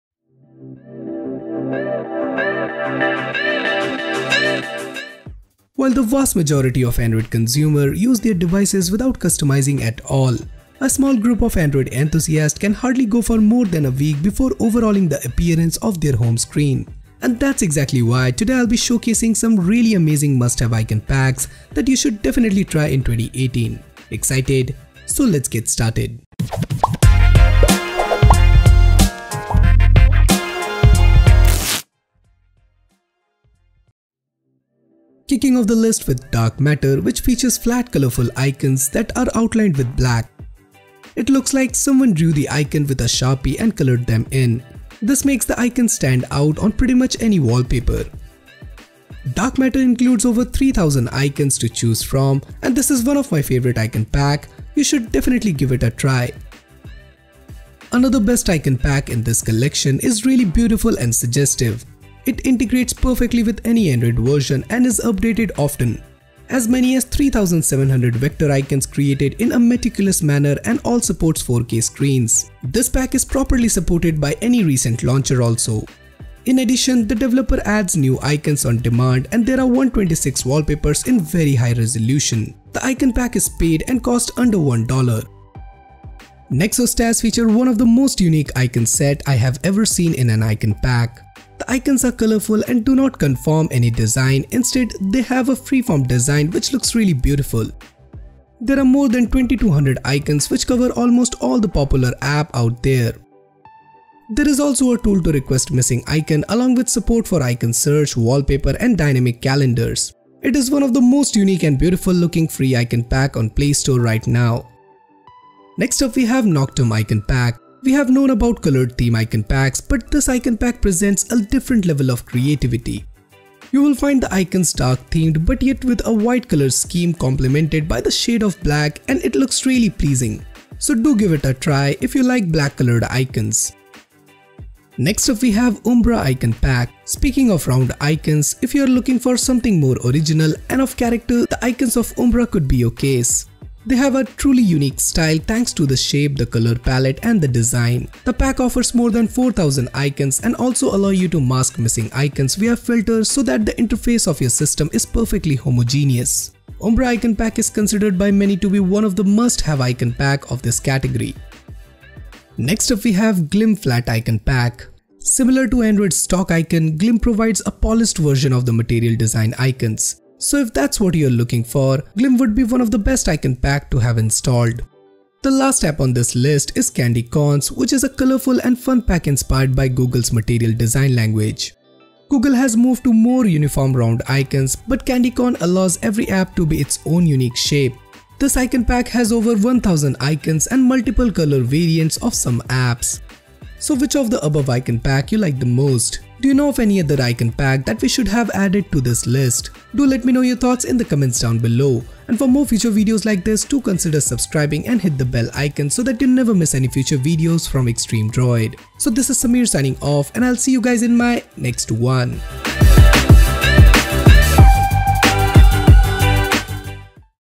Top 7 MUST HAVE Icon Packs | Best Icon Packs 2018
While the vast majority of Android consumers use their devices without customizing at all, a smaller group of Android enthusiasts can hardly go for more than a week before overhauling the appearance of their home screens.

And that’s exactly why today I will be showcasing some really amazing must have icon packs that you should definitely try in 2018.

[Downloads]